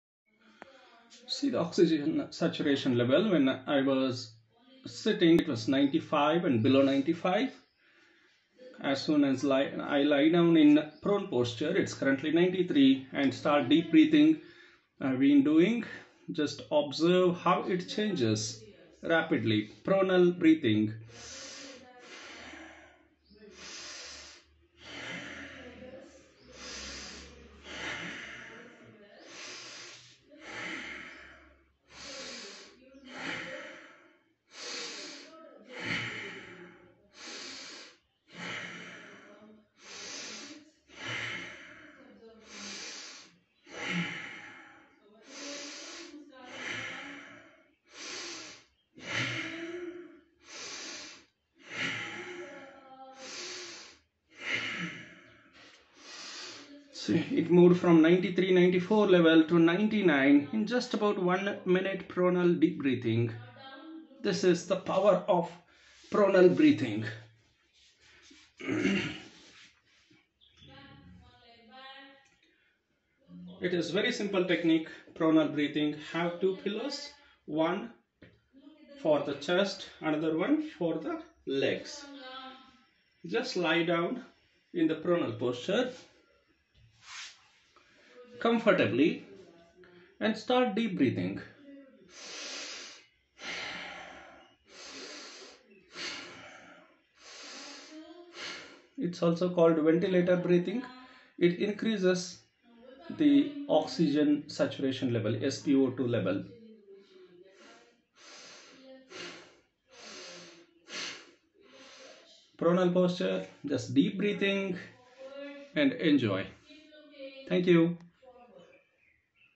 Pronal or ventilator breathing. See the amazing results .
Pronal or ventilator breathing.
See the amazing results .
Follow this simple technique to increase the oxygen levels in a couple of minutes...